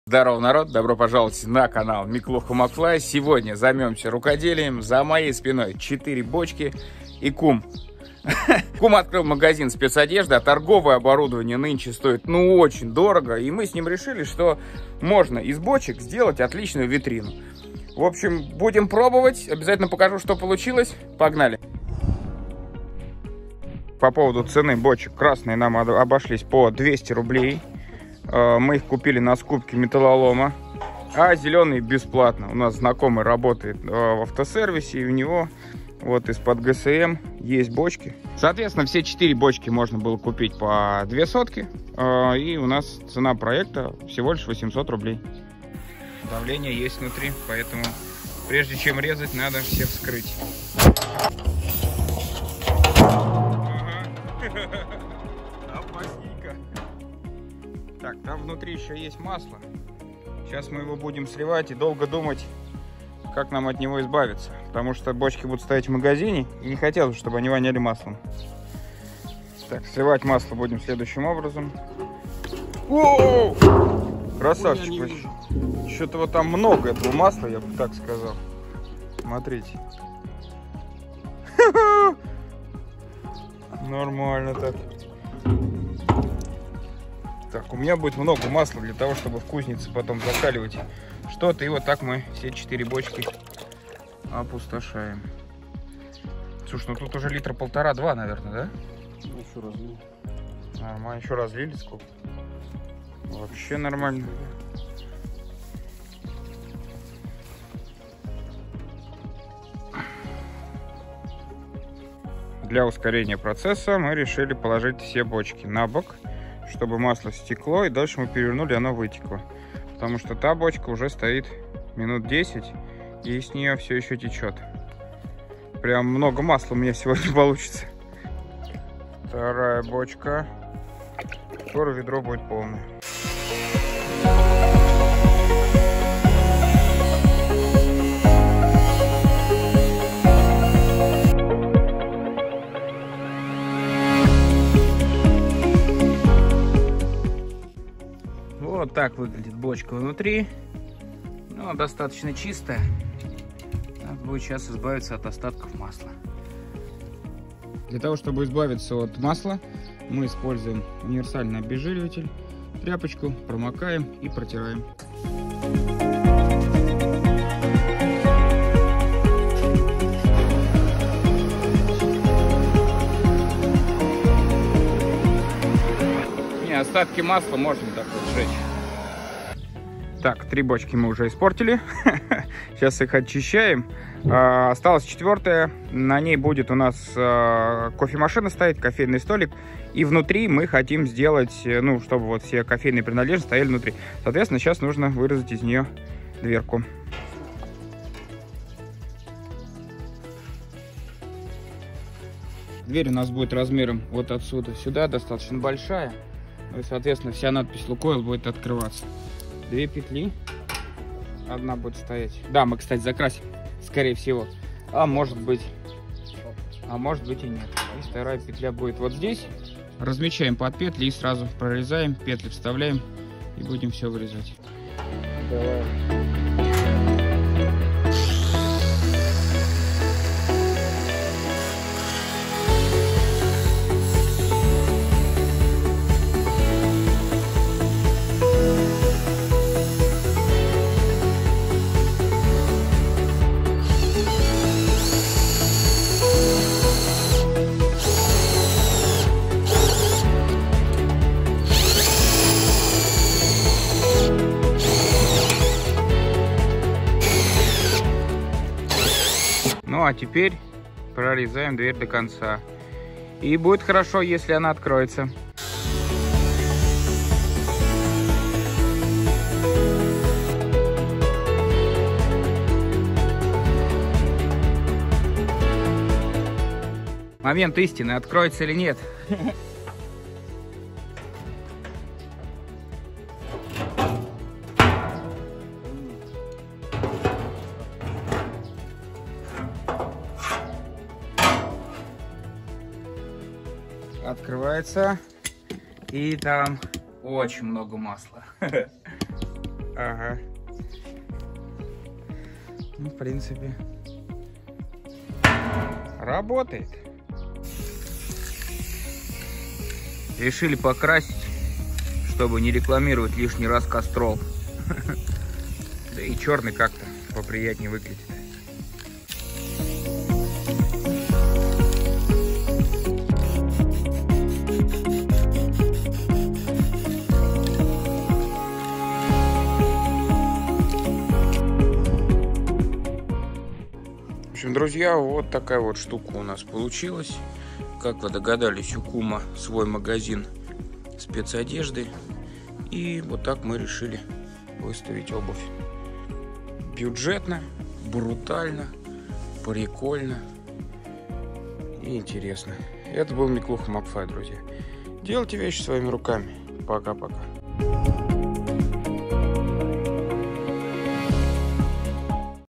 Самоделка из старых бочек. Сэкономили с кумом кучу денег и сделали мебель своими руками
Кум открыл свой магазин спецодежды и решили мы сделать торговое оборудование своими руками. Купили две 200 литровые бочки, еще две нам подарили. А что в итоге получилось смотрите в этом видео.